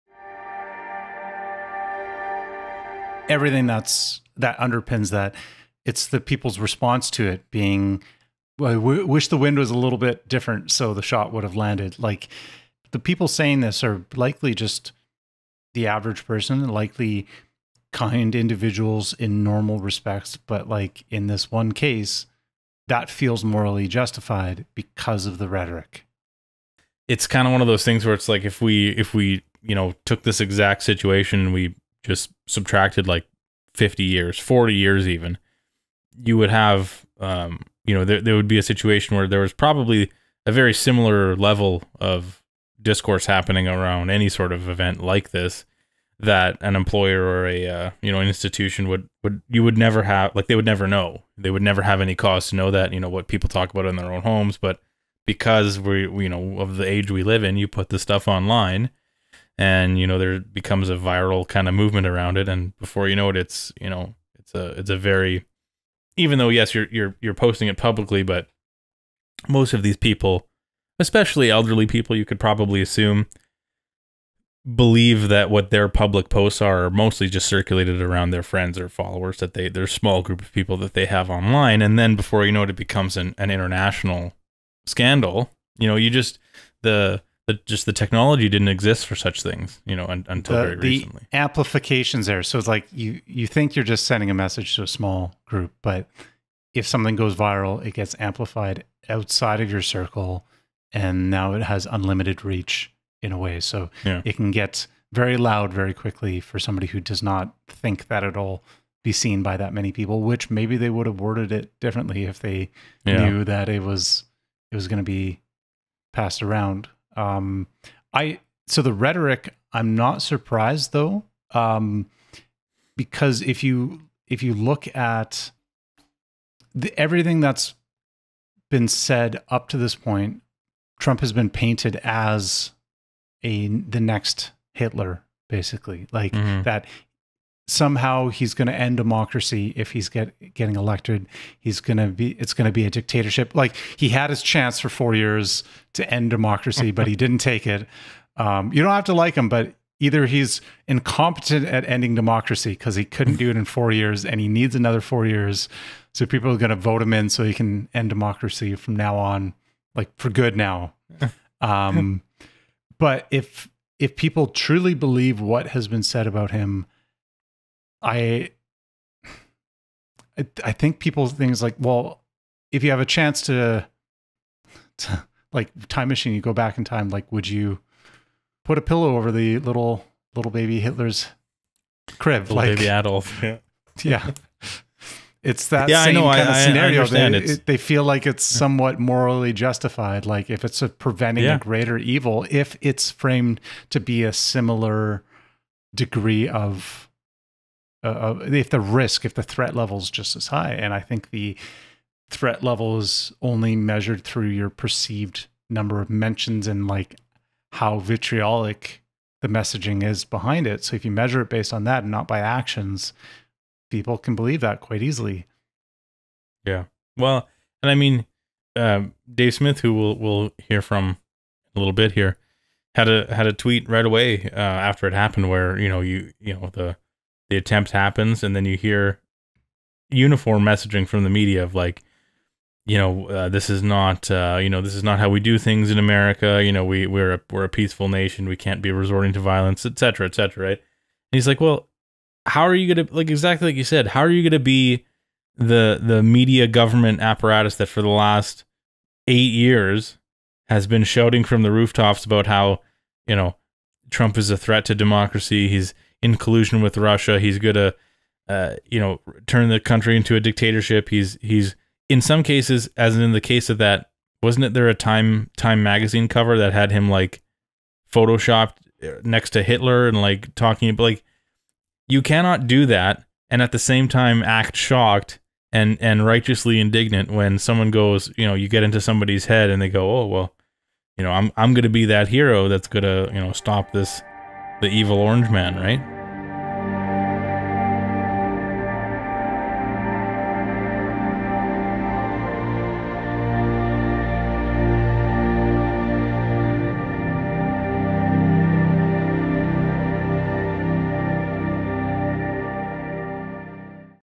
"I Wish He Didn't Miss" | A Critical Compass Clip
Does the potential for amplification and virality in today's social media landscape change the way we should approach commenting on political scandals? And does the media bear any responsibility for those scandals due to some of the rhetoric they deploy against their political enemies?

***Note on what Mike said at the end of this clip: It was actually The New Republic which ran a cover featuring a photograph of Trump stylized in the style of 1932 H*tler campaign poster. The post can still be viewed on X